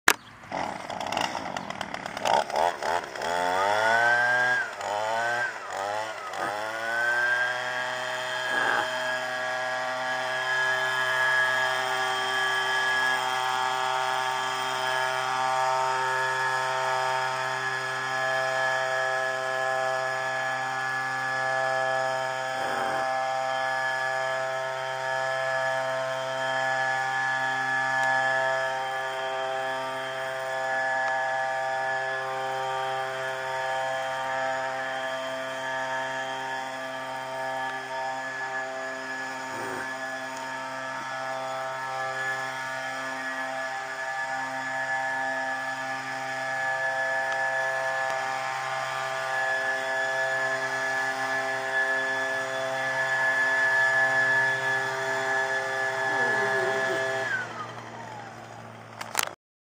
Echo (PAS) And Blower Attachment
(5/21/11) The articulating hedge trimmer is awesome, along with the Weedeater, and Blower... so far ! Awesome set up !!